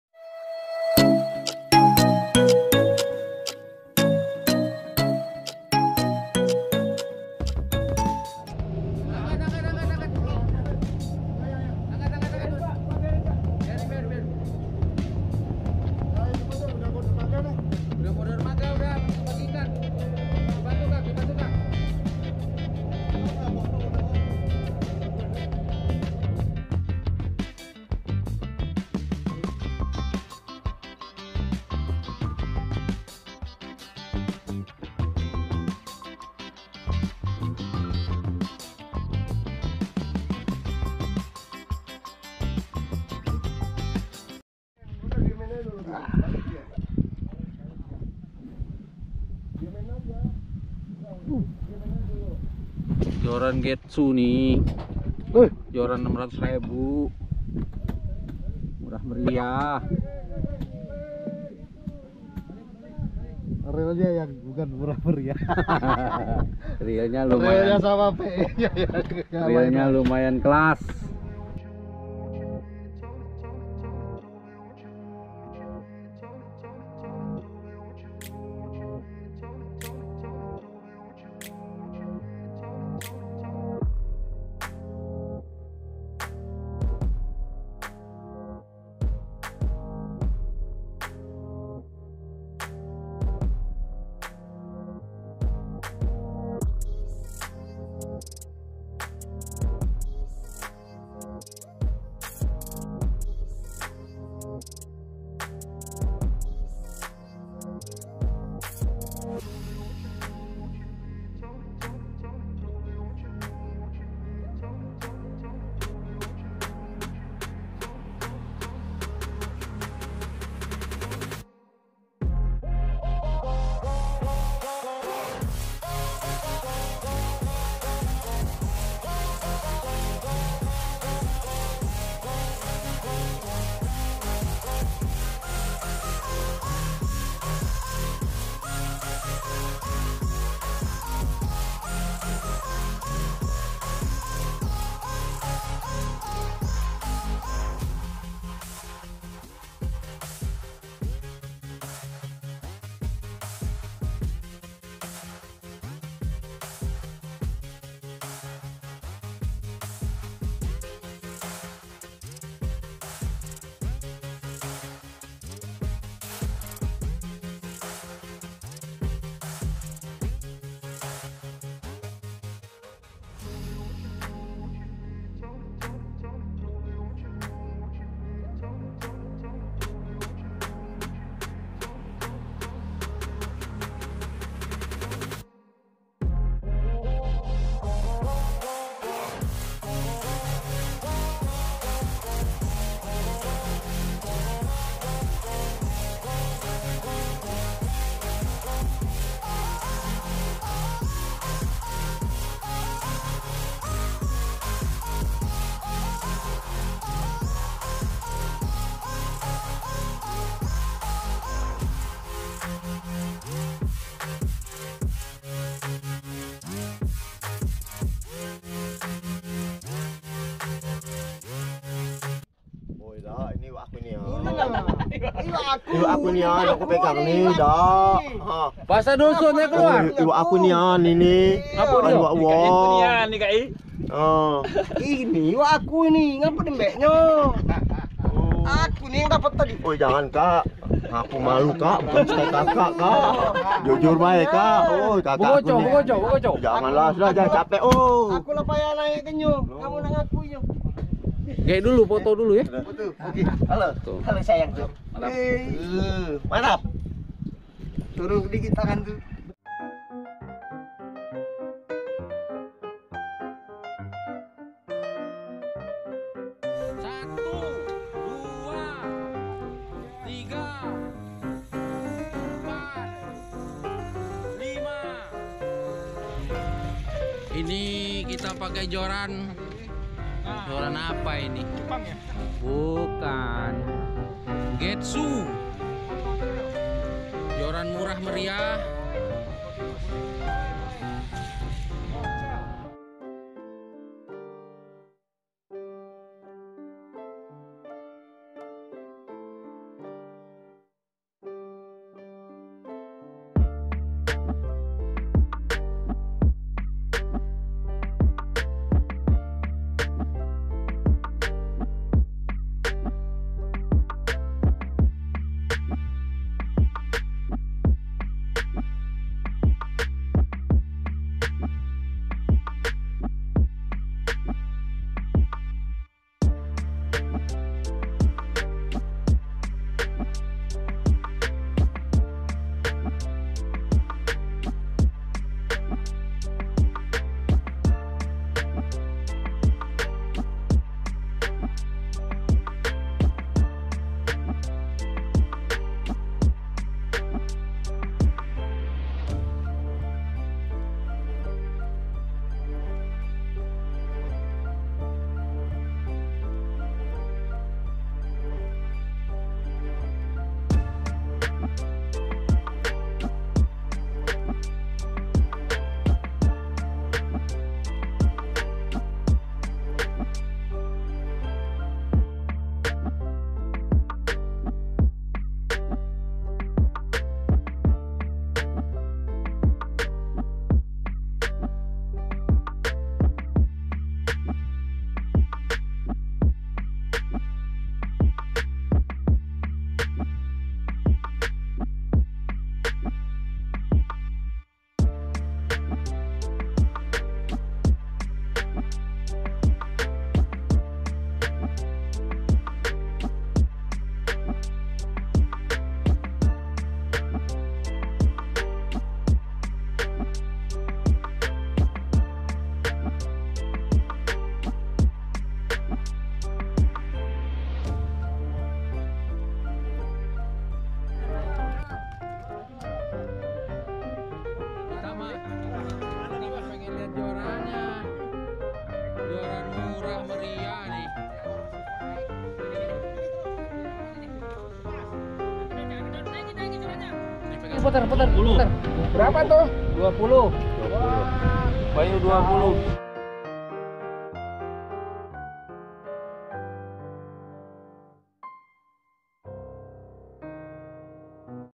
ROD POPPING GETSU BERULAH LAGI
Hai sobat mancing!

Rod murah meriah bisa strike giant trevelly babon, simak keseruan 2 angler yang mencoba sensai strike dengan rod GETSU

Selamat menonton 😄🙏